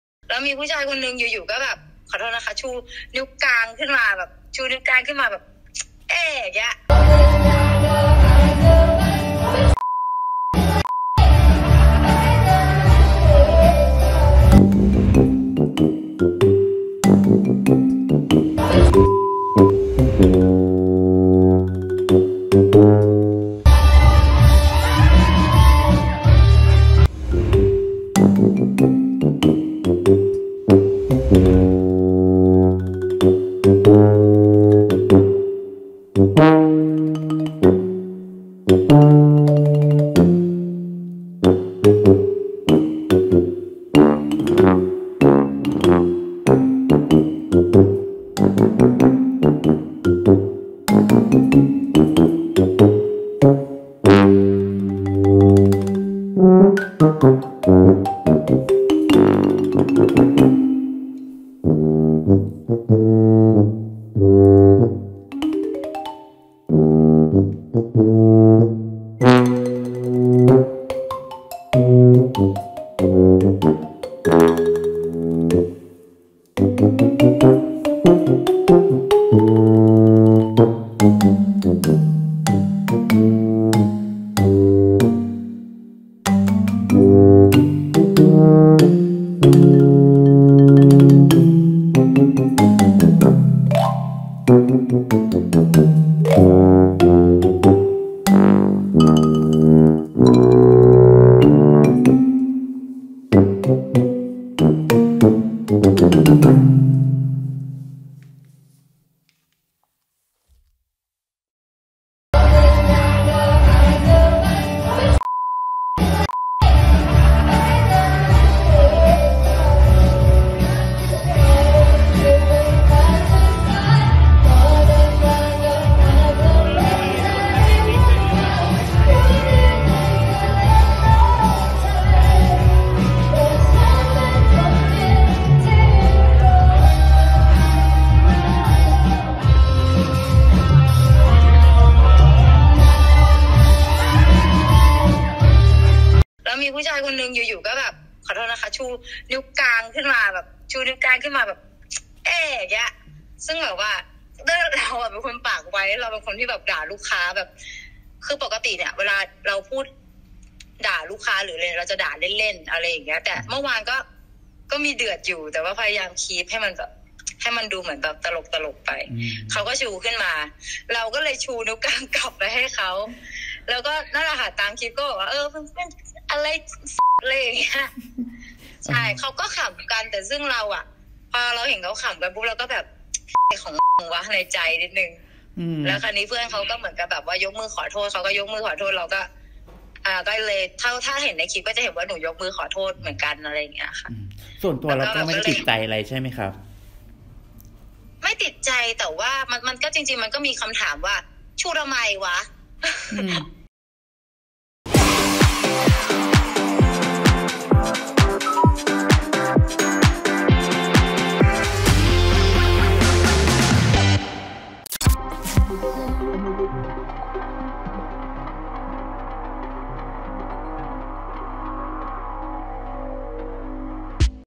เมื่อลูกค้าชูนิ้วกลางให้บนเวที ก็เจอนักร้องสาวด่ากลับ แถมตั้งการ์ดสับเข่าพร้อมชก | ข่าวทันกระแส
เมื่อลูกค้าชูนิ้วกลางให้บนเวที ก็เจอนักร้องสาวด่ากลับ แถมตั้งการ์ดสับเข่าพร้อมชก